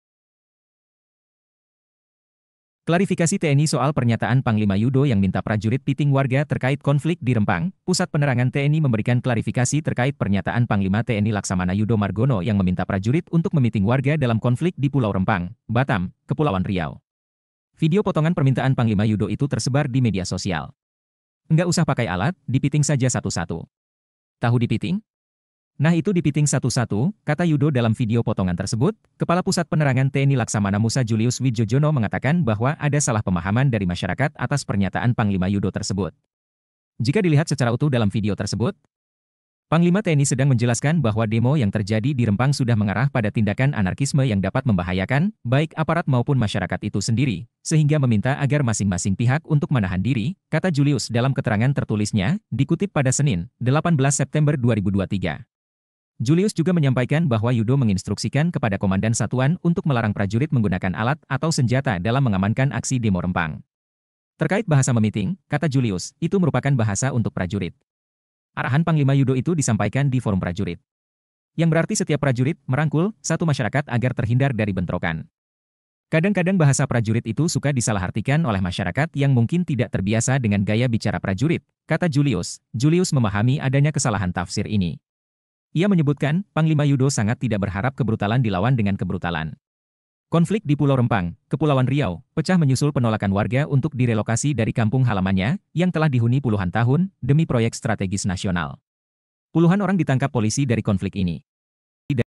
KLARIFIKASI TNI soal Pernyataan Panglima Yudo yang Minta Prajurit “Piting” Warga Rempang
Pusat Penerangan TNI memberikan klarifikasi terkait pernyataan Panglima TNI Laksamana Yudo Margono yang meminta prajurit untuk “memiting” warga dalam konflik di Pulau Rempang, Batam, Kepulauan Riau.
“Enggak usah pakai alat, dipiting saja satu-satu. Tahu dipiting? Nah itu dipiting satu-satu,” kata Yudo dalam video potongan tersebut.